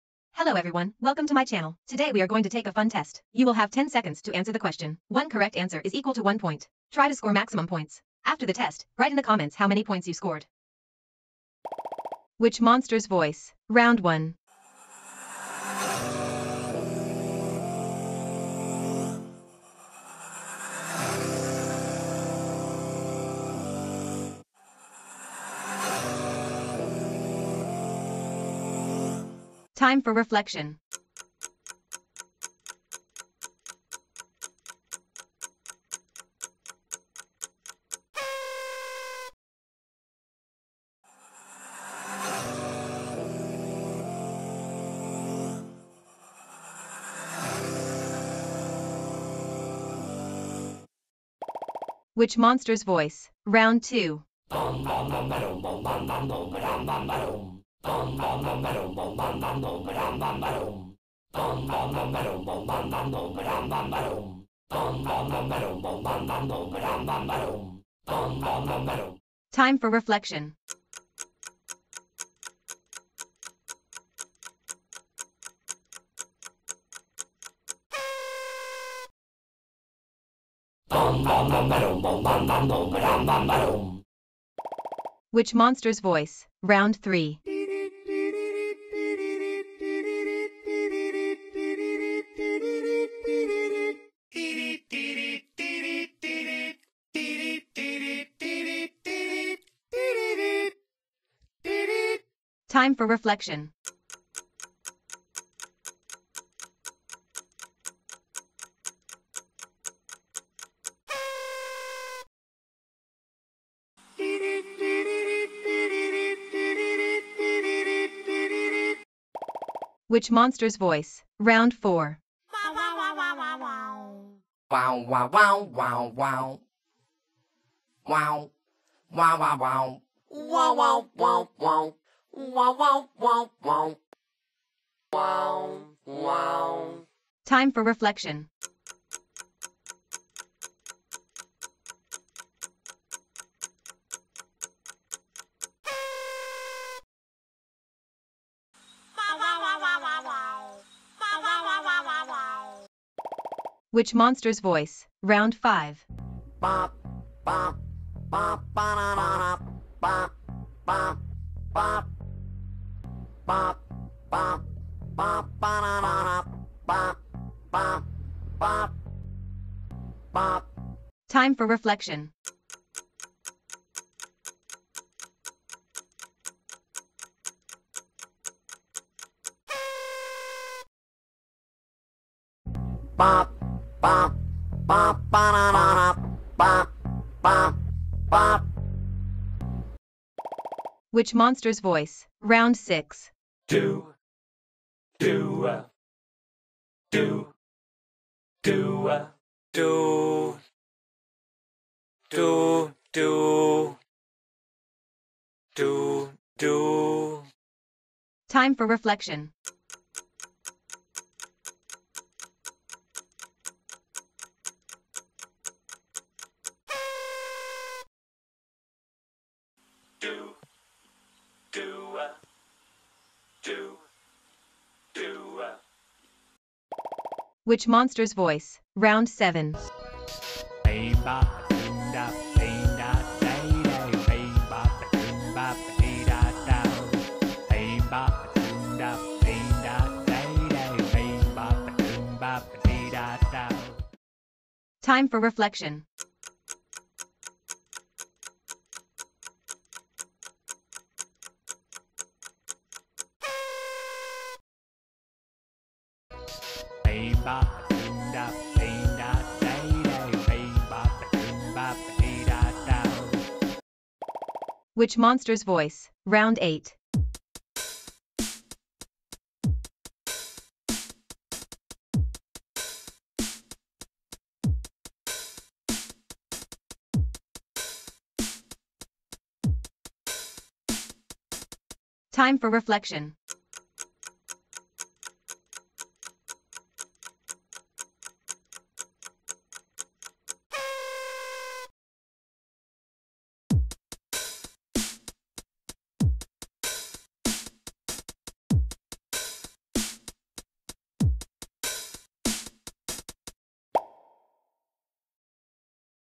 GUESS MONSTER'S VOICE #78| Monculus, Dermit, Maw, Fwog, Potbelly, Toe Jammer, Jellbilly, Repatillo
Get ready for a monstrous adventure with Monculus, Dermit, Maw, Fwog, Potbelly, Toe Jammer, Jellbilly, and Repatillo! Join us as we dive into the world of Guess the Monster Voice, where these unique characters come to life. Discover their enchanting voices, uncover their hidden talents, and test your monster knowledge. Can you guess which voice belongs to each quirky creature? Tune in and prepare to be entertained by the captivating world of these lovable monsters in Guess the Monster Voice!

I have 30 new interesting Monsters video for next 30 days ❤️